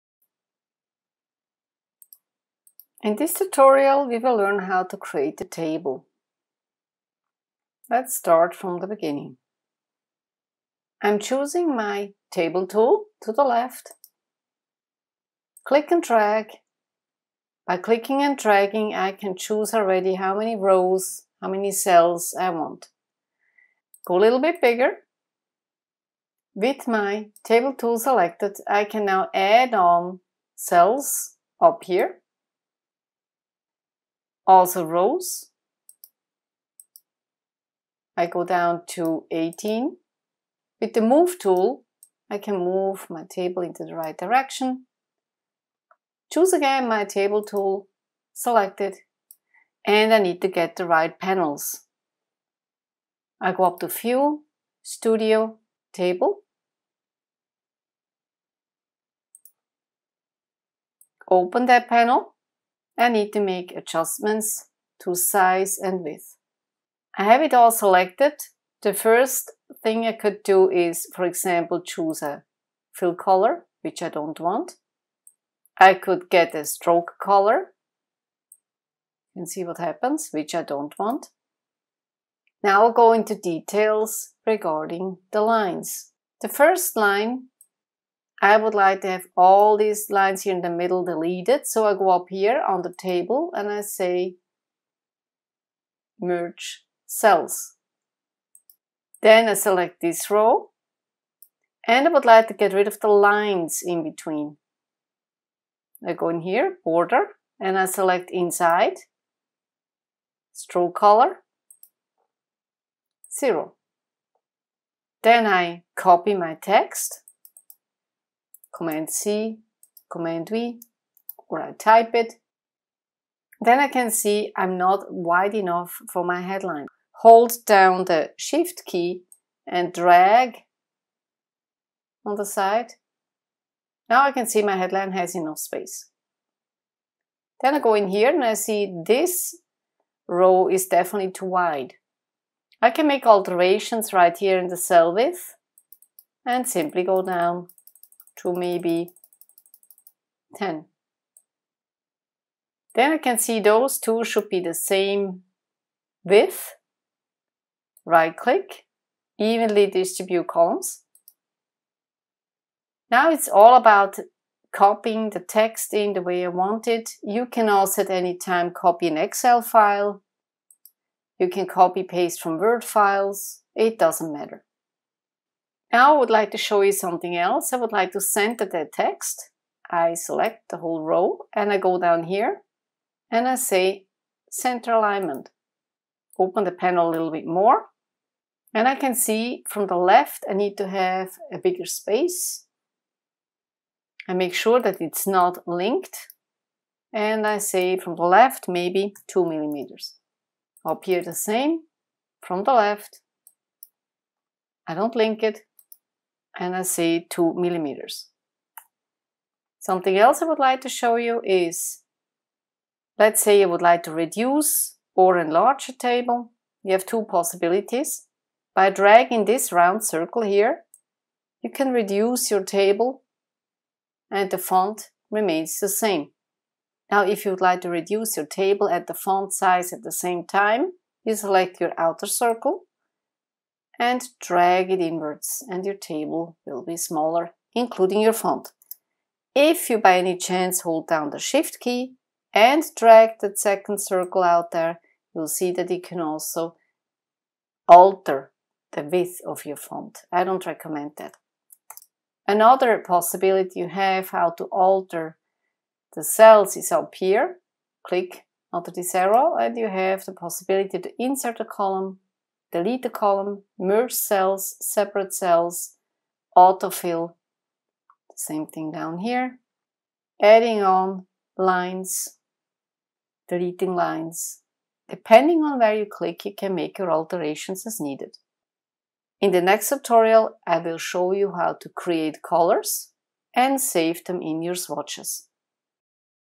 How to create a Table with AFFINITY PUBLISHER
How to create Tables with Affinity Publisher. It's never been so easy to create tables - check out our free Affinity & Adobe tutorials at i-adobe.com.